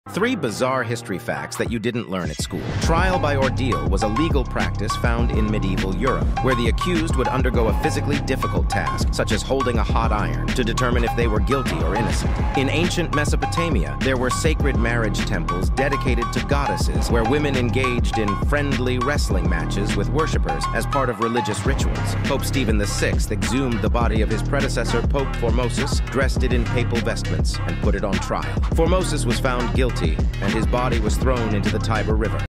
Crazy History Facts School Didn't Teach You! - Part 6
Dive into the eccentricities of ancient civilizations with these shocking historical practices! Discover the intense trials of medieval Europe, where guilt or innocence was determined through grueling ordeals. Witness sacred Mesopotamian rituals where women engaged in "friendly wrestling matches" in devotion to their goddesses. Uncover the jaw-dropping Cadaver Synod, as Pope Stephen VI puts his predecessor, Pope Formosus, on trial in an eerie posthumous spectacle. Join us on this journey through the weird and wonderful annals of history! 🕰️🔍